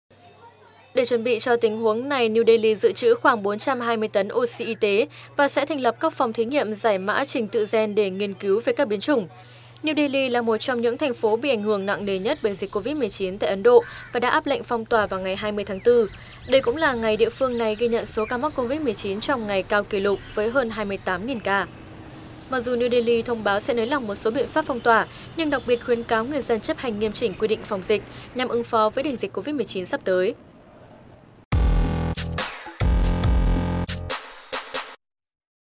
Ấn Độ chuẩn bị đối mặt với đỉnh dịch
Thủ đô New Delhi của Ấn Độ đang chuẩn bị đối mặt với đỉnh dịch COVID-19, với khoảng 37 nghìn ca mắc mới mỗi ngày.